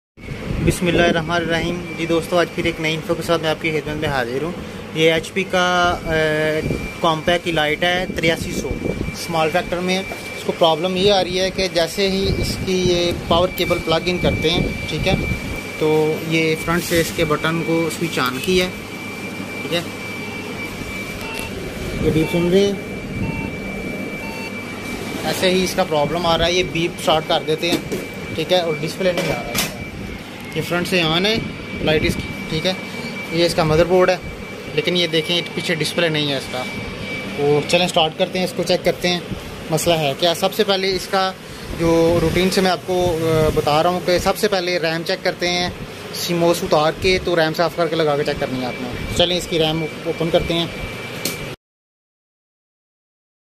HP Compaq 8300 3 Beep problem
Hello friend's

mostly help full video for review's and repairing and information and informative and interesting

HP Compaq EliteDesk 8300
3 Beep problem
clear this problem